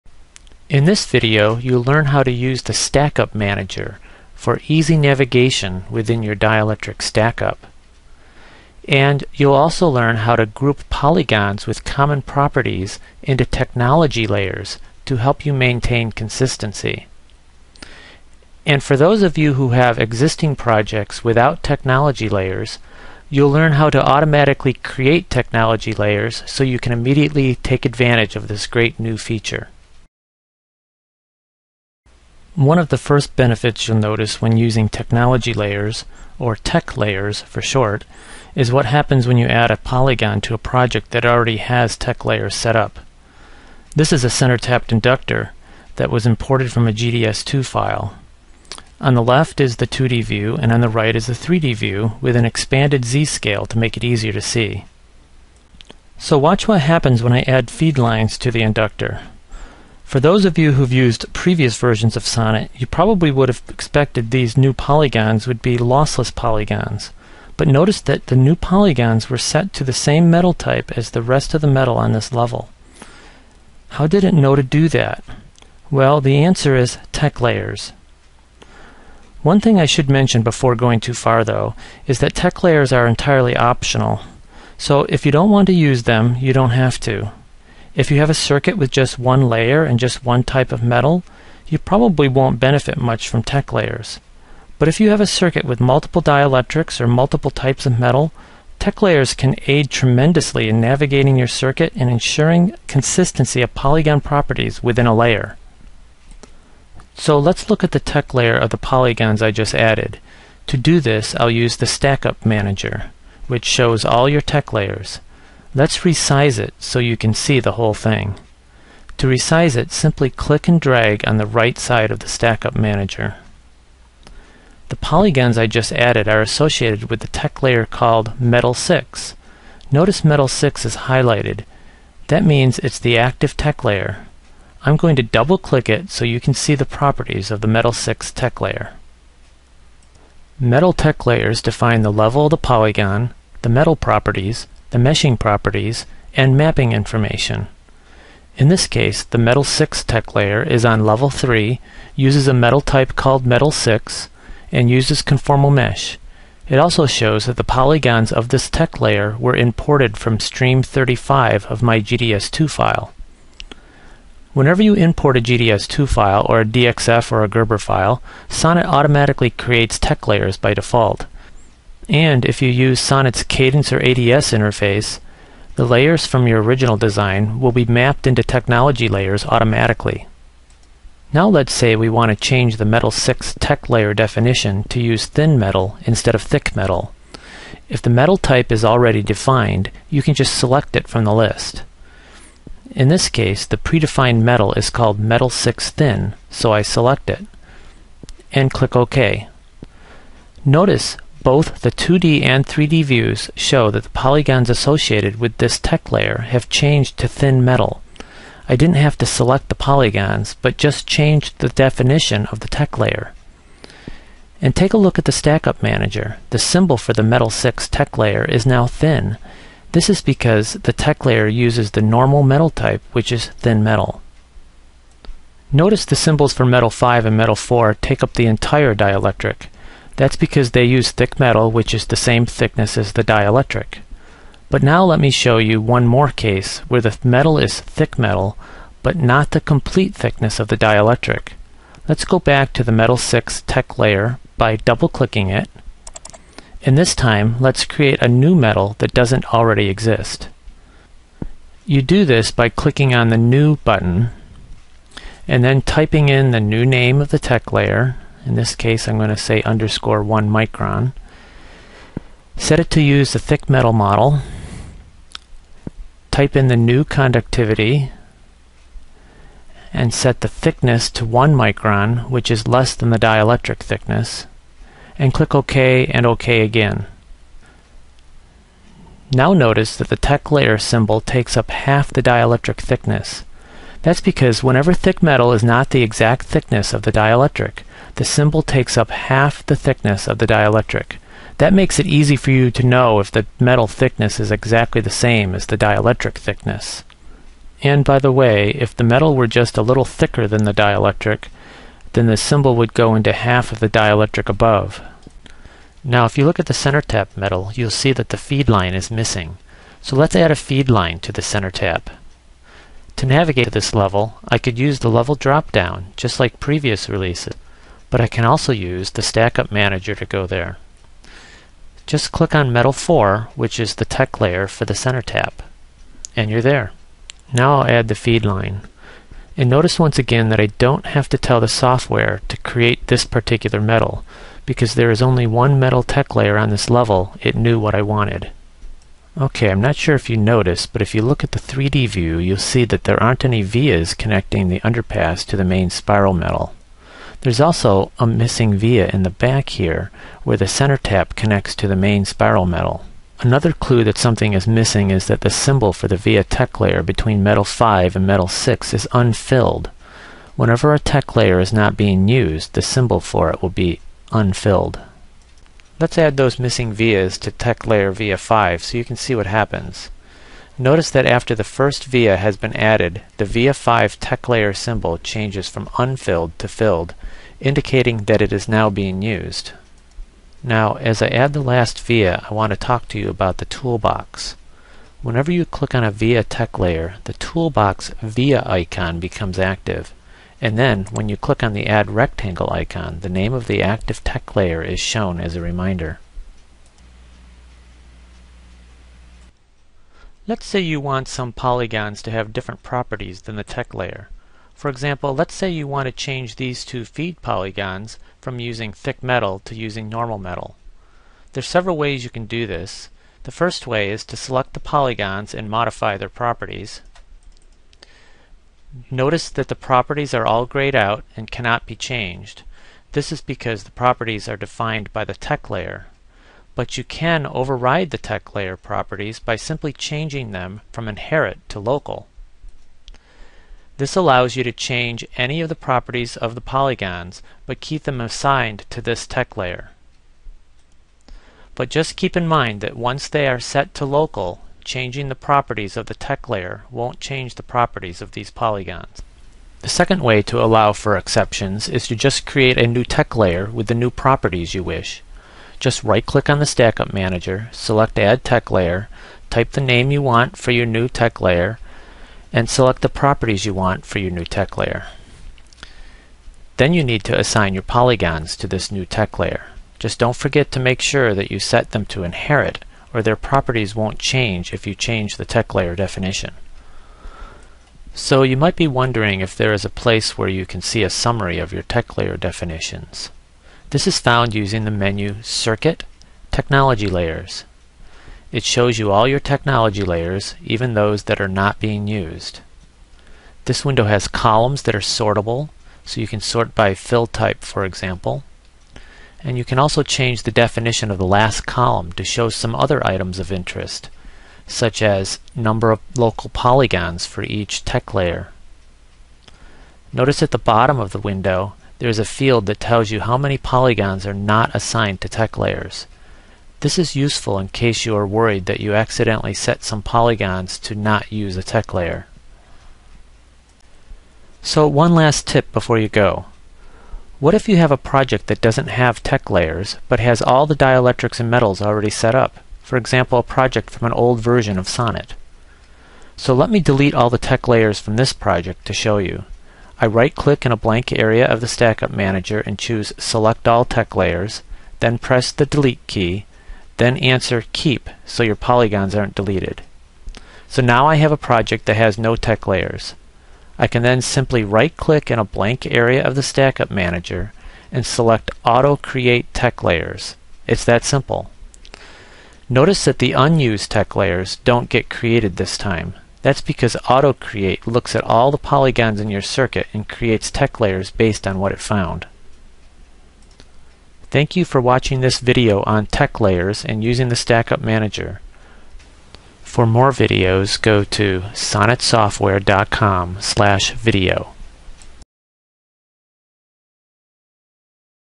Technology Layers (Older version of Sonnet)
This video gives you an overview of the Stackup Manager and Technology Layers.
This video was made with an old version of Sonnet and may not be relevant when using the latest version.